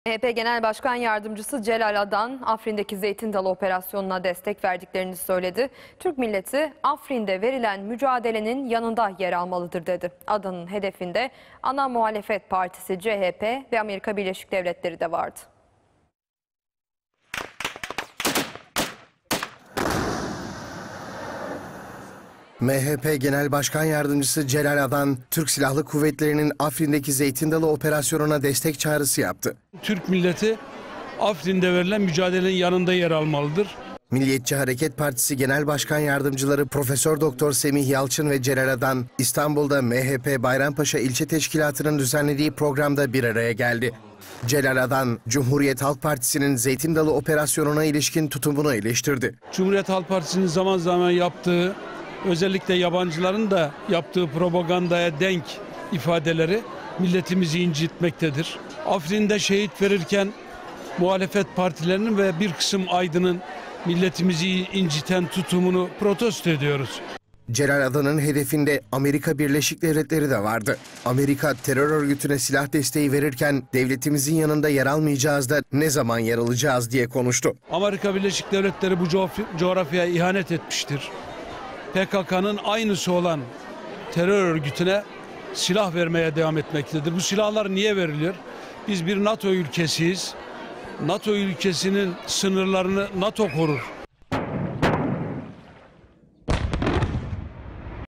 Muhalefet partilerinin ve bir kısım aydının milletimizi inciten tutumunu protesto ediyoruz.
Celal Adan: Afrin'de Şehit verirken muhalefet partilerinin ve bir kısım aydının milletimizi inciten tutumunu protesto ediyoruz.